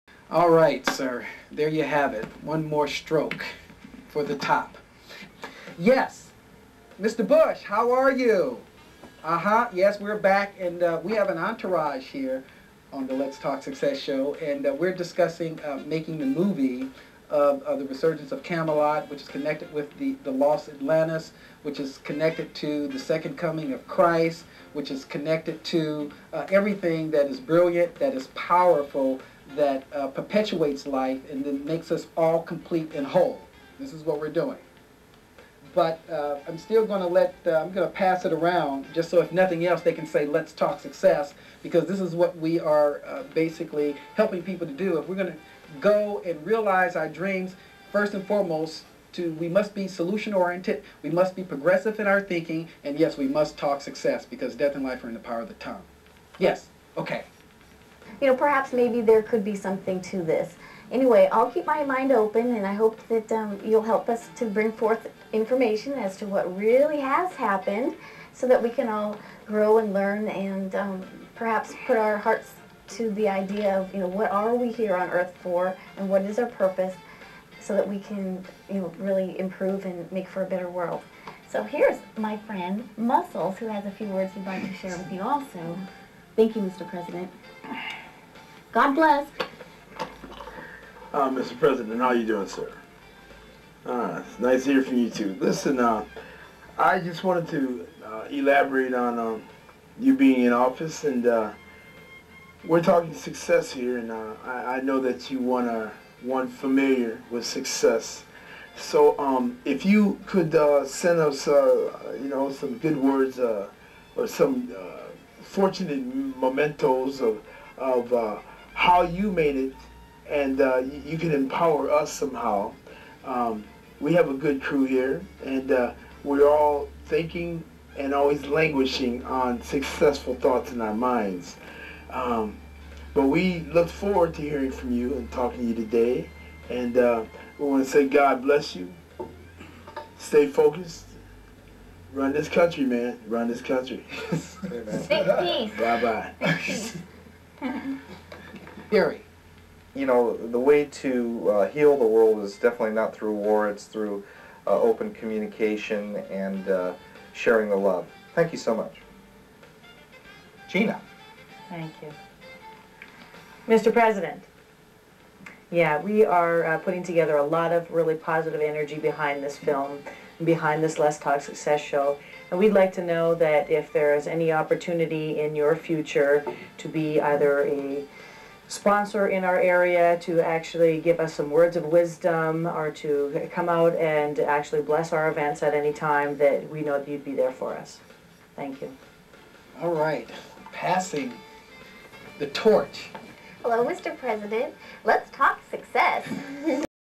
WHW Facebook White House
"Special Events" at 'Let's Talk Success".org production team has a wireless phone conversation
with the president in an attempt to get him involved. This
is for the WHW(West Hollywood Wing Reality Show) on Facebook